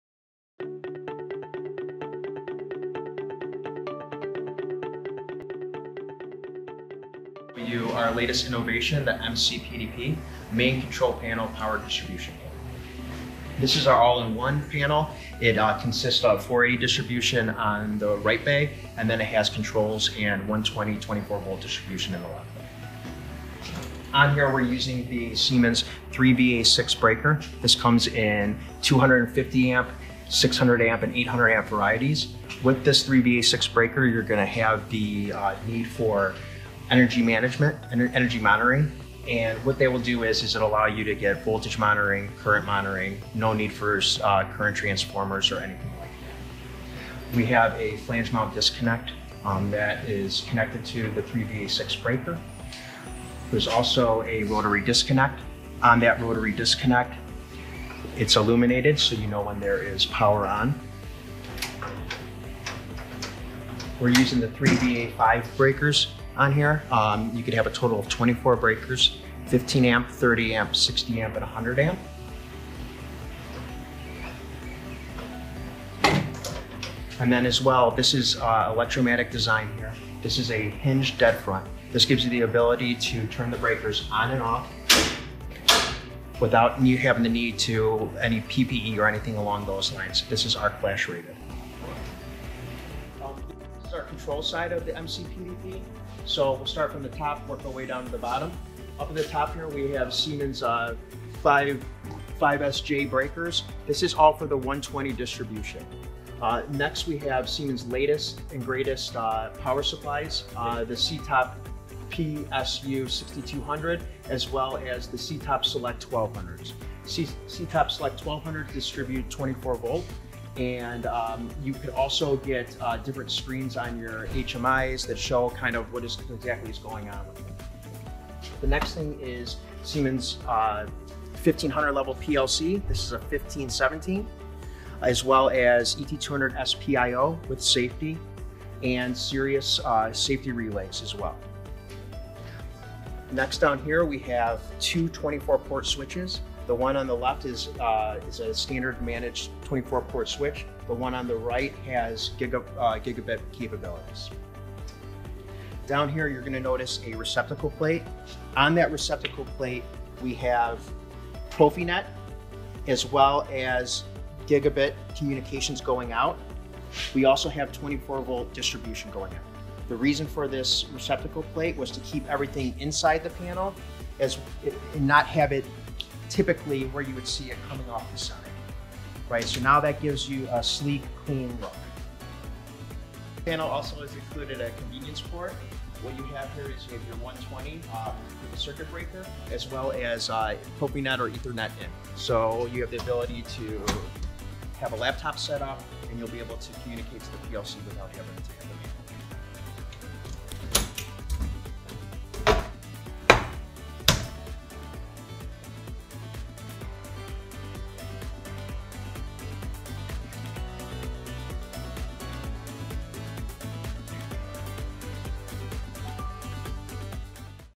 Power Distribution Panels
Electro-Matic’s Power Distribution Panels (PDP’s) stand above any other solutions out there. Our competitively priced PDP’s offer many advantages including customizable designs. Work with our experienced engineering team to discuss your needs and assist you in determining the best solution for your application.